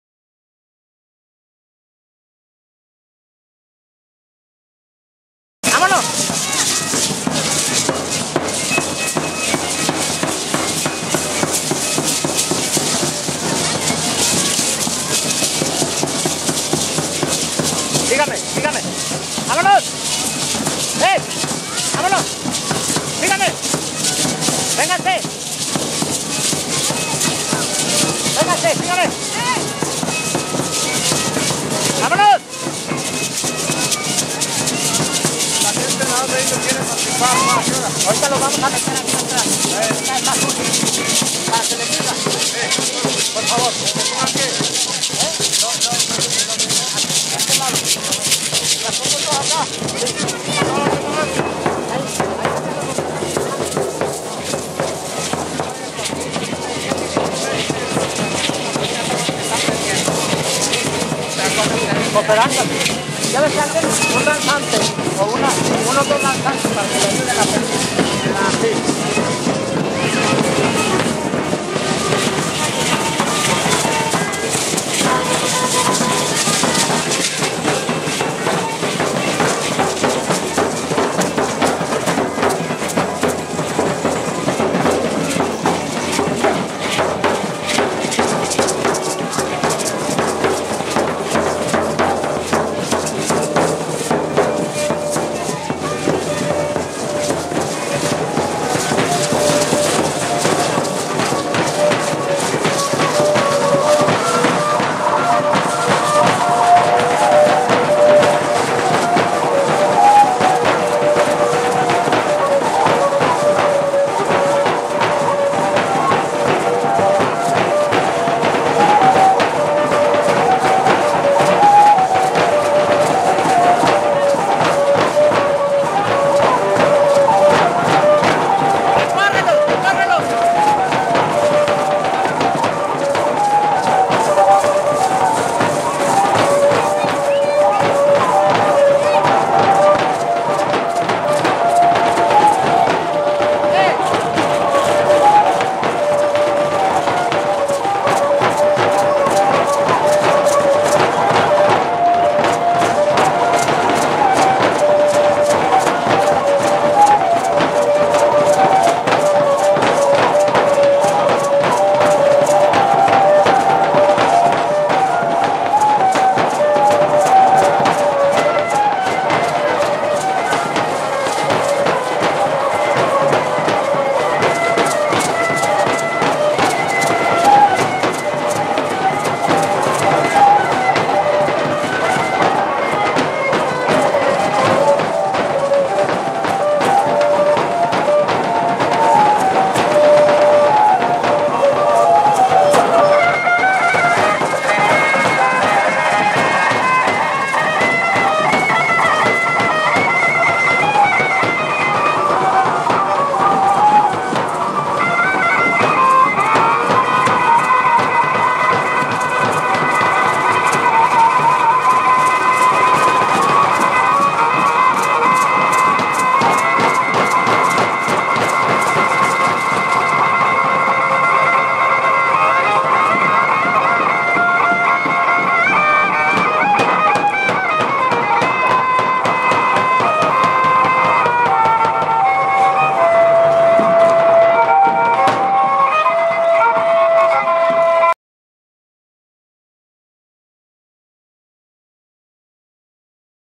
Recibimiento a Esmeralda, Dgo.
5° Encuentro de Danzas, Música y Canto de La Laguna. Ejido 20 de Noviembre, Coah.  a 25 de septiembre de 2011.